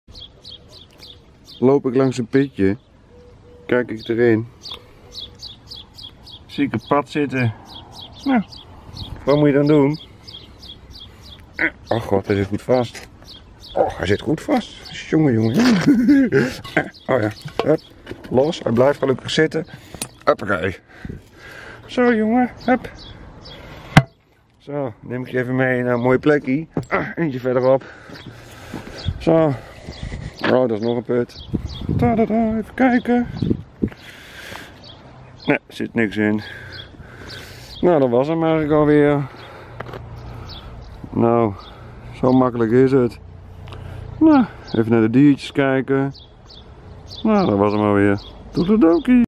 Pat uit put gered. Zo makkelijk gaat dat. Honden bezitters kijk eens in een put tijdens je wandeling
EU&VN@AntieFauna . Dieren hebben hulp nodig. Geef hun jouw stem.